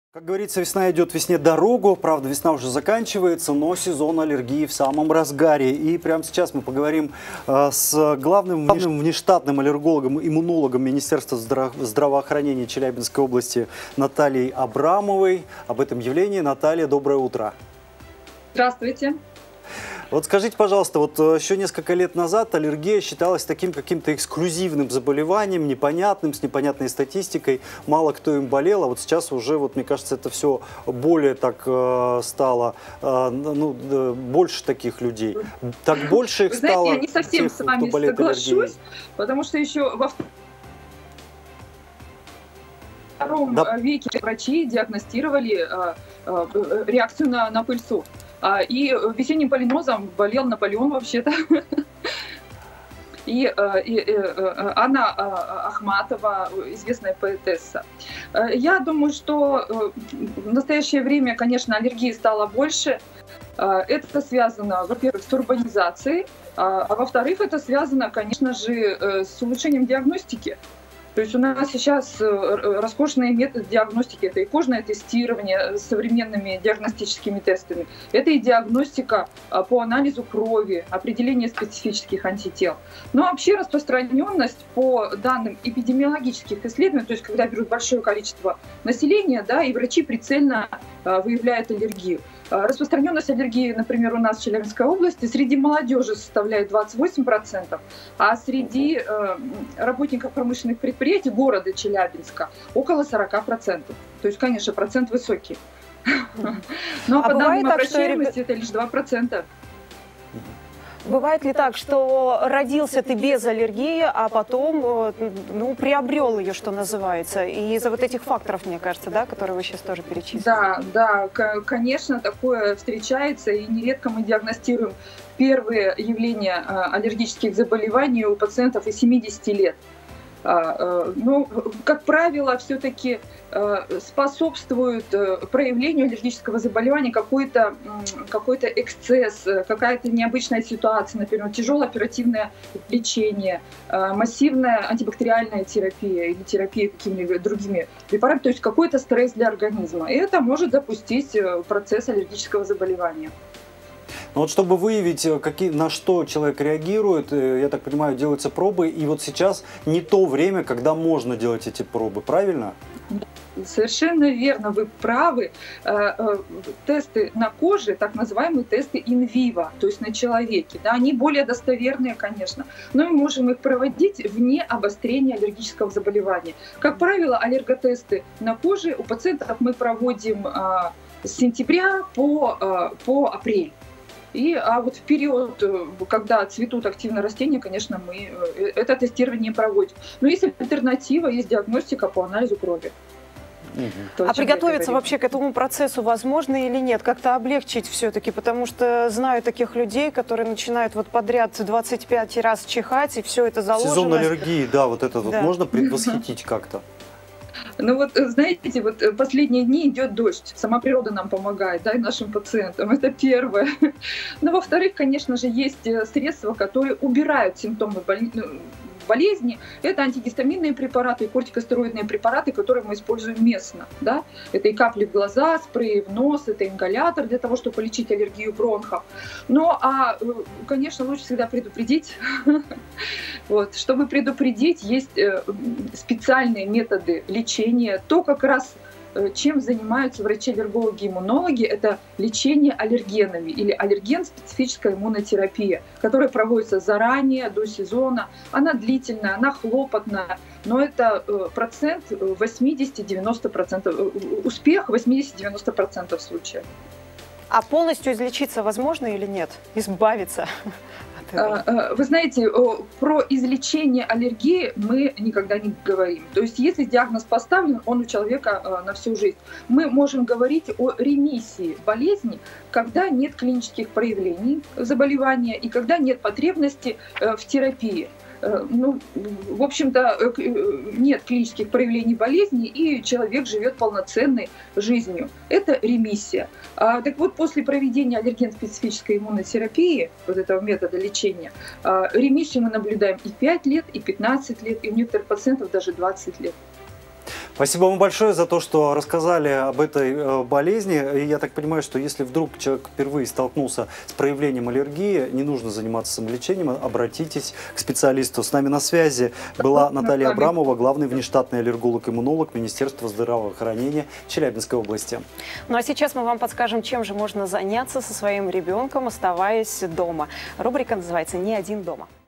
Гость на связи — как спастись от аллергии
Май — прекрасный месяц, когда природа полностью оживает после долгой южноуральской зимы. На деревьях распускаются почки, а затем и листья, на зелёных газонах набухают бутоны цветов. И если для кого-то май — это время для прогулок в тени деревьев и фотосессий на фоне яблонь и сирени, то для других — по настоящему тяжелый период из-за аллергии.

Чиханея, насморк, покраснения глаз — лишь малая часть тех симптомов, которые испытывают люди с аллергией на цветение растений. Но можно ли как-то подготовиться к этому периоду, чтобы аллергия не беспокоила весной или вовсе вылечиться от этой болезни. мы узнали у Натальи Абрамовой, главного внештатного аллерголога-иммунолога Министерства здравоохранения Челябинской области.